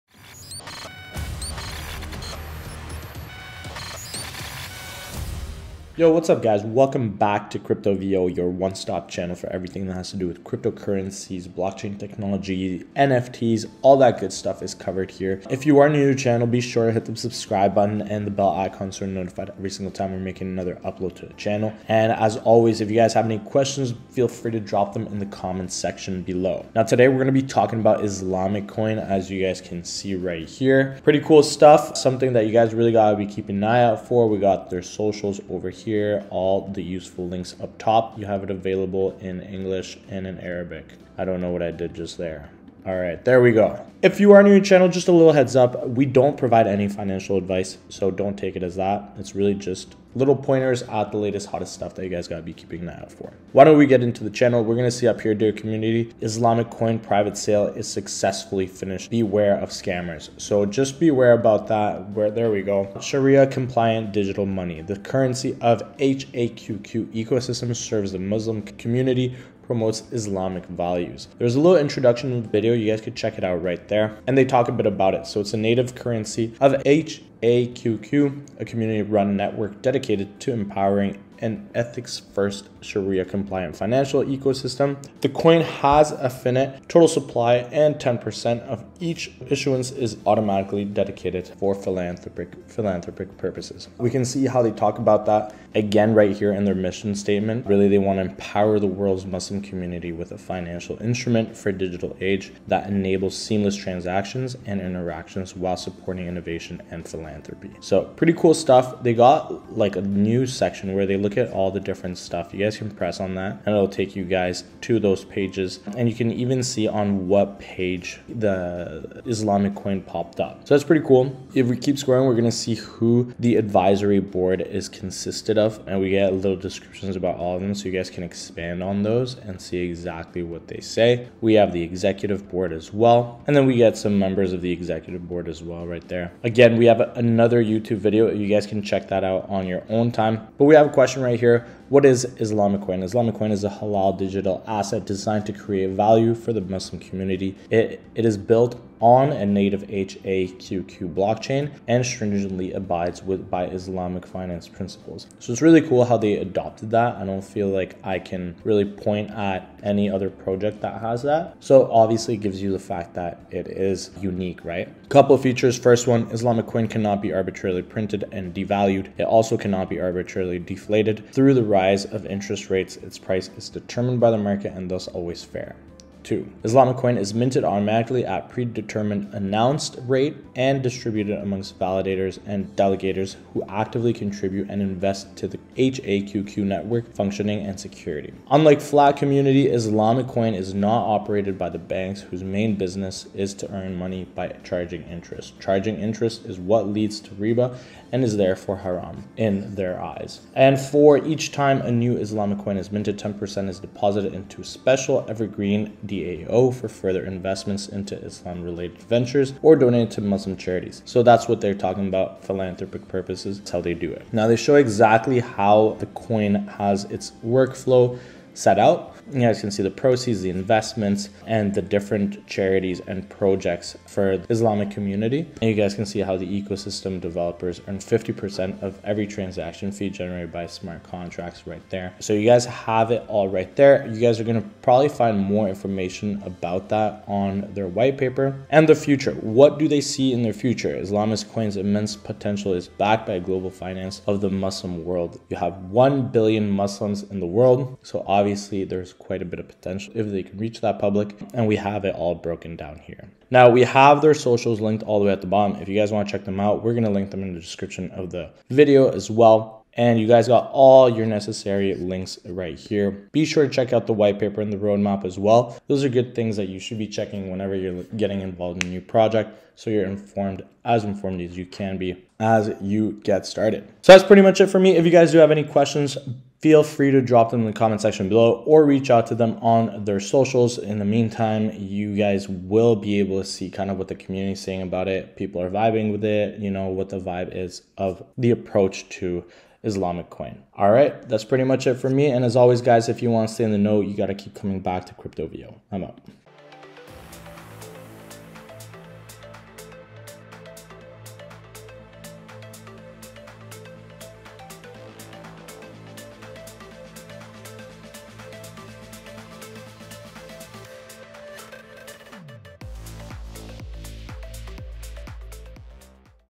Islamic Coin - The next big step for Islamic Finance - AIRDROP CONFIRMED
To enter the chance of winning a 500 ISLM coin airdrop, download the Haqq Wallet app using the following

Register your wallet and share your Haqq Wallet address in the comment section below!